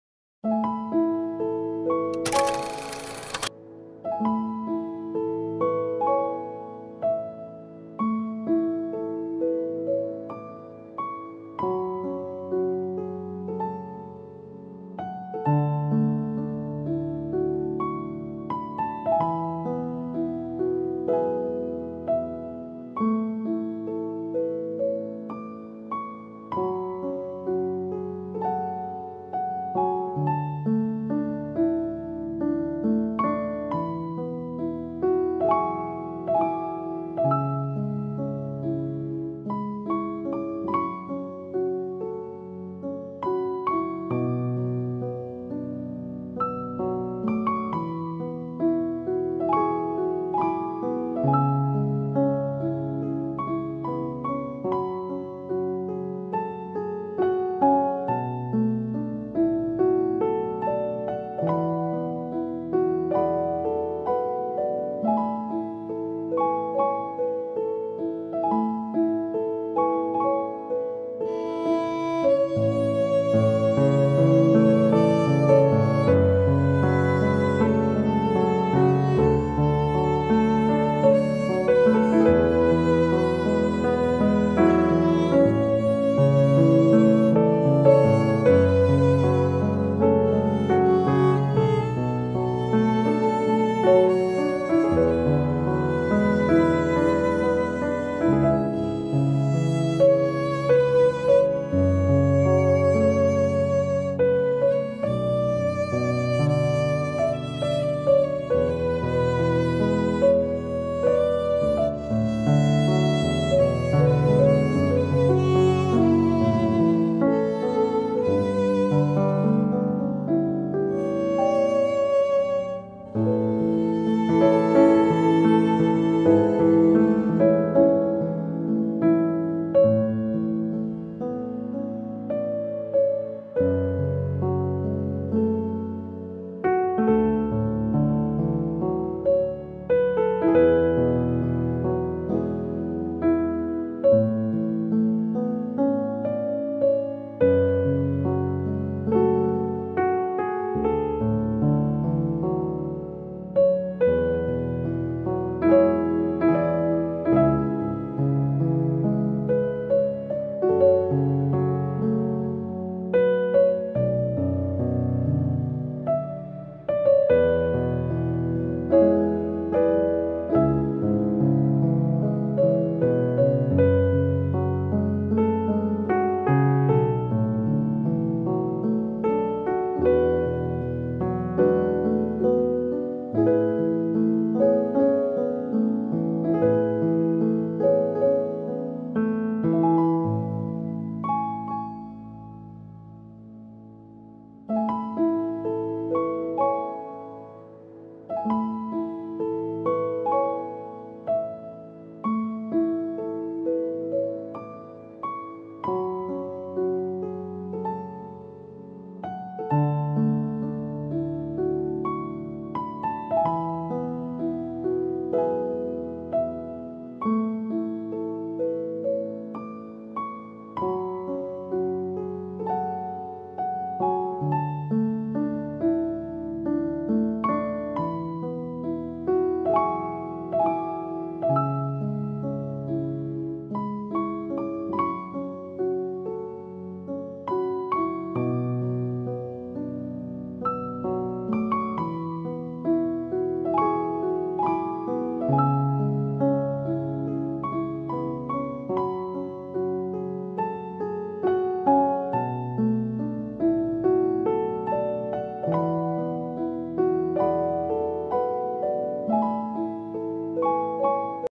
Beautiful Paper Wall Hanging Craft | Paper Craft For Home Decoration |DIY Wall Decor| Easy Wall mat
New Idea Wall hanging Craft Very Easy
Wall hanging for beginners